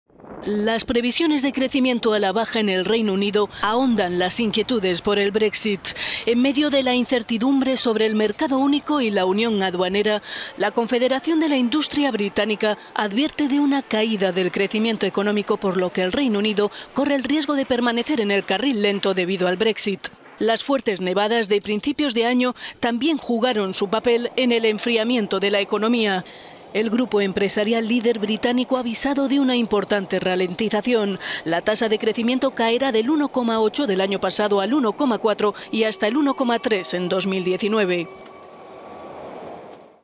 Negras perspectivas para la economía británica
Las previsiones de crecimiento a la baja en el Reino Unido ahondan las inquietudes por el Brexit.…